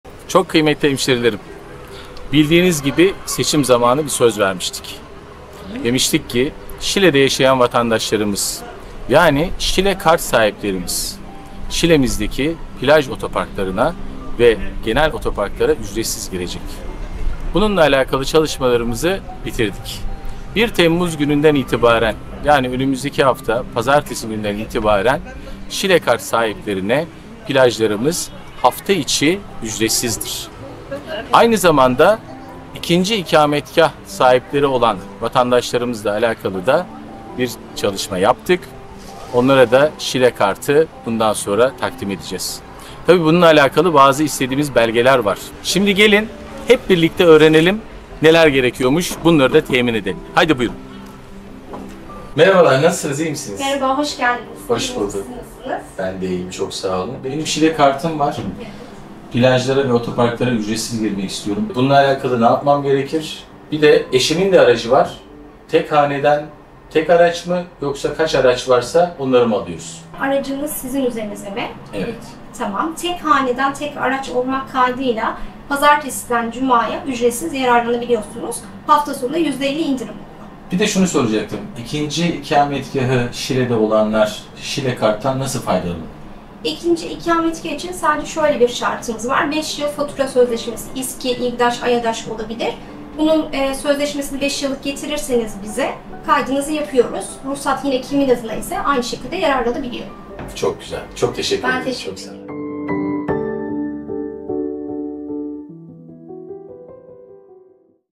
Müjde! Söz verdik, yapıyoruz!
📢 Şile Kart sahibi ve ikinci ikametgahı Şile olan hemşehrilerimize Müjde!

1 Temmuz itibari ile Şile Kart sahibi hemşehrilerimizi bekleyen avantajlar:

✅ Şile Kart sahibi hemşehrilerimiz, araç ruhsatı kart sahibi adına olmak kaydıyla her haneden 1 araç olacak şekilde, otopark ve plaj girişlerinden hafta içi ücretsiz, hafta sonu ise %50 indirimli faydalanabilecekler.

✅ İkinci ikametgahı Şile olan hemşehrilerimiz de Şile Kart sahibi olabilecekler.

📌 İkinci ikametgahı Şile olan hemşehrilerimiz Şile Kart'ı nasıl alabilirler? 👇🏻

🗒️ İkinci ikametgahı en az 5 yıl ve üzeri süre Şile’de olan hemşehrilerimiz, 5 yıllık fatura sözleşmesi (İSKİ, AYEDAŞ, İGDAŞ vb.) ruhsat belgesi ve TC vatandaşlık kimlik belgesi ile Şile Kart ofisimize başvurarak Şile Kart sahibi olabilirler.
Şile Kart’a özel yenilikler devam edecek!